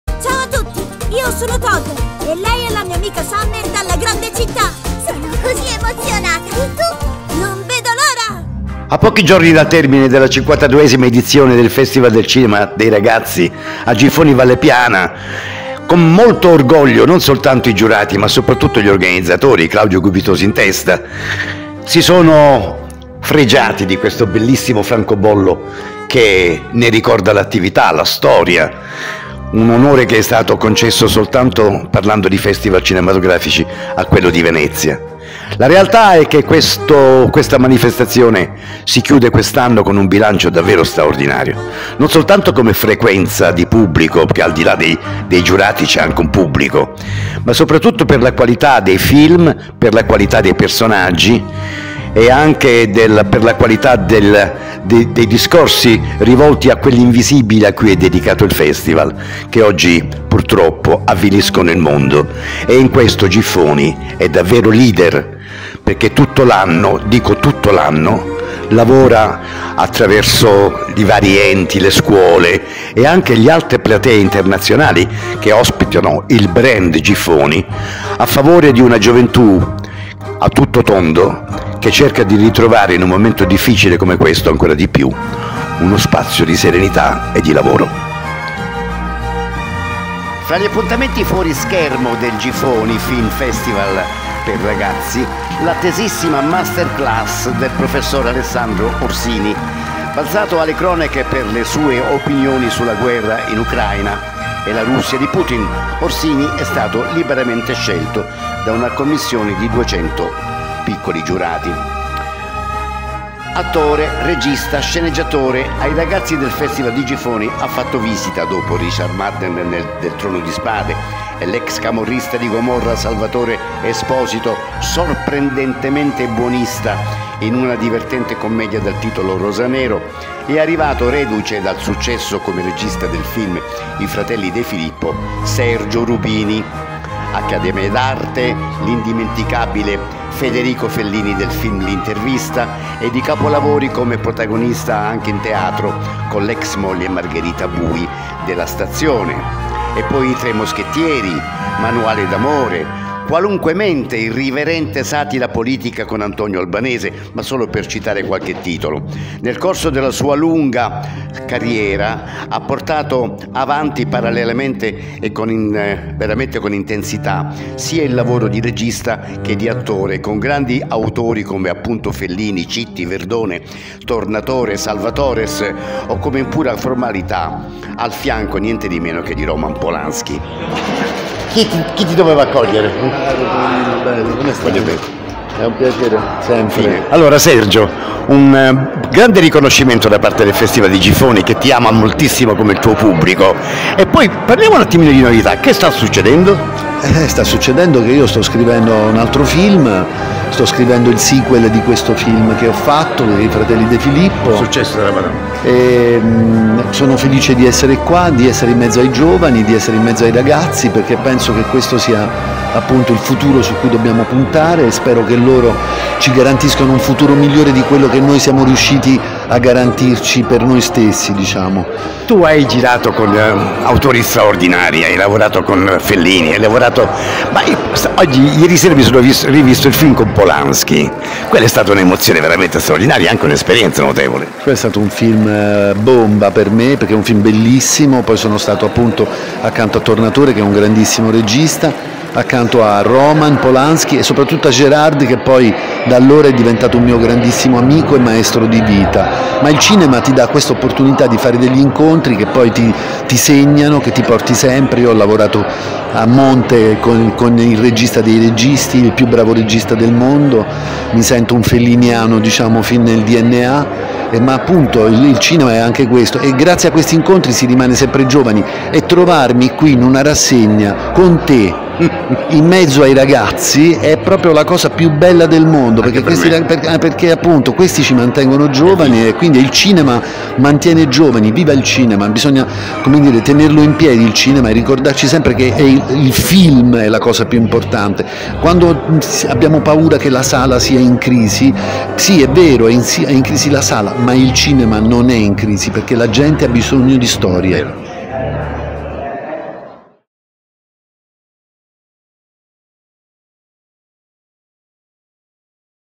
Giffoni Film Festival - 7a giornata
Tonino Pinto dal Festival di Giffoni ci fa il resoconto della giornata introducendo il pezzo forte del servizio ovvero l'intervista a Sergio Rubini.